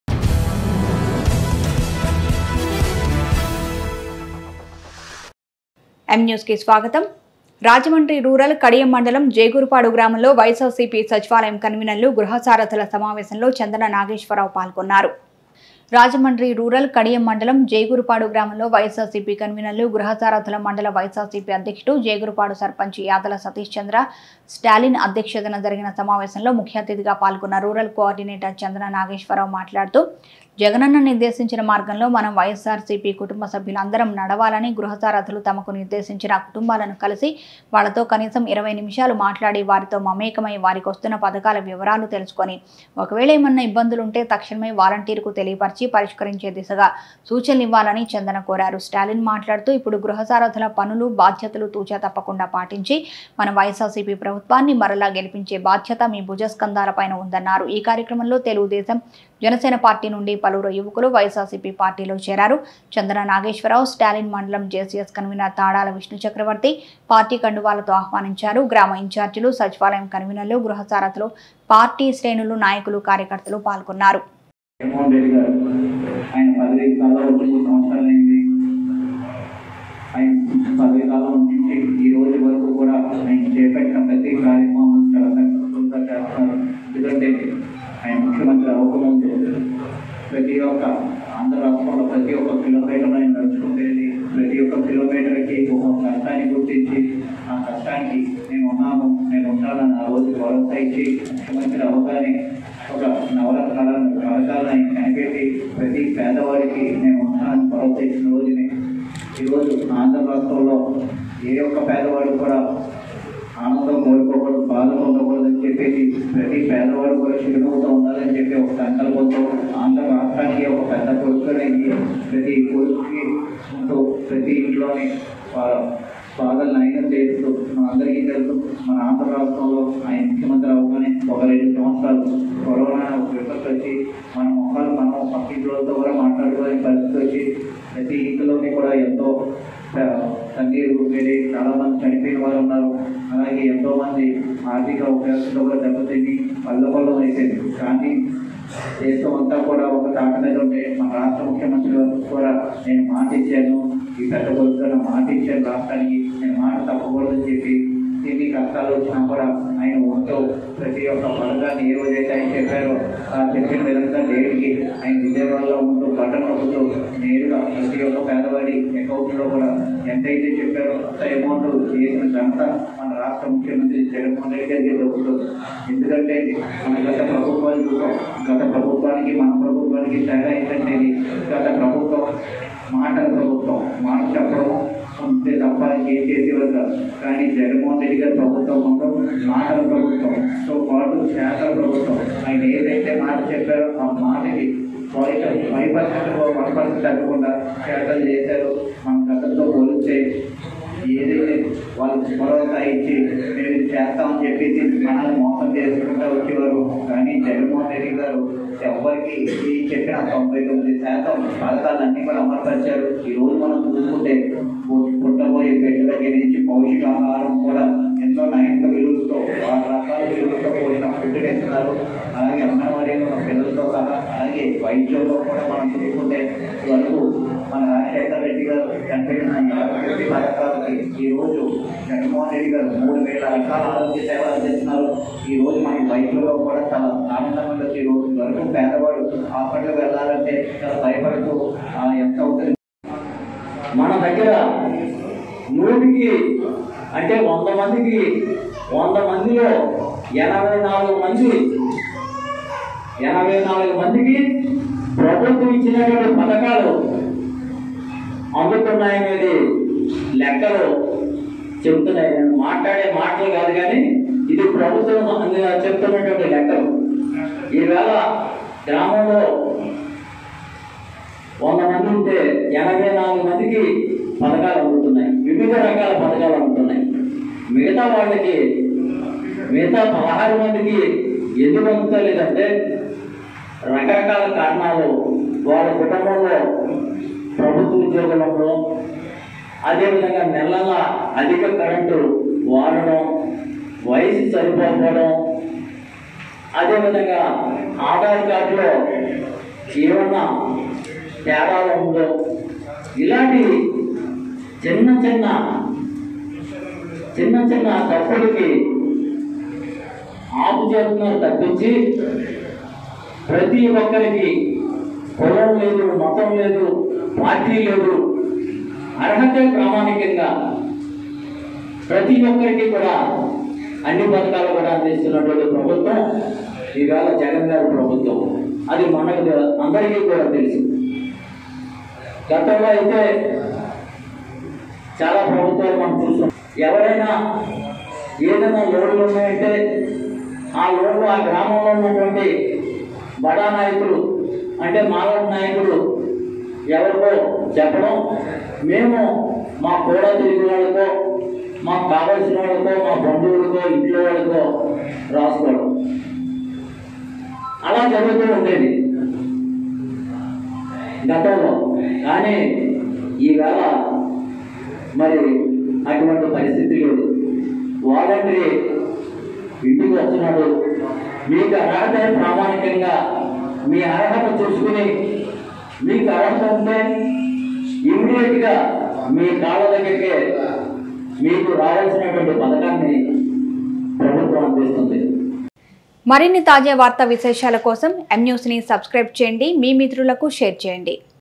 జేగురుపాడు గ్రామంలో వైస్సార్సీపీ సచివాలయం కన్వీనర్లు, గృహ సారధుల సమావేశం||M NEWS TELUGU
రాజమండ్రి రూరల్, కడియం మండలం ,జేగురుపాడు  గ్రామంలో వైస్సార్సీపీ సచివాలయం కన్వీనర్లు, గృహ సారధుల సమావేశంలో చందన నాగేశ్వర్ పాల్గొన్నారు||M NEWS TELUGU